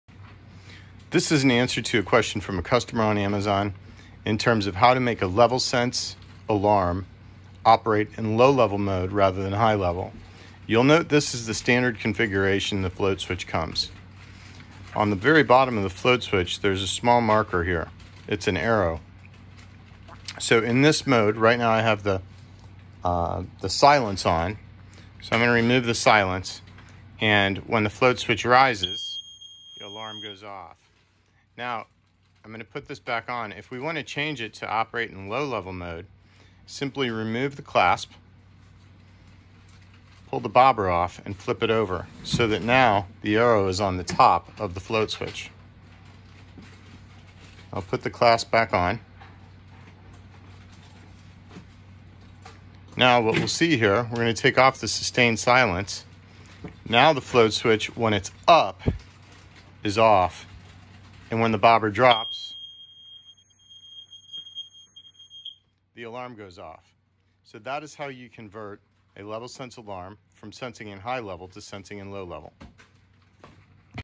How to Change Level Sense Alarm to Alert on Low Level and Alarm when a Tank or Sump Is Empty
The standard Level Sense Alarm will Alert when it is in High Level Mode. This video shows how to invert the operation to Detect A Low Level in a Cistern or Tank.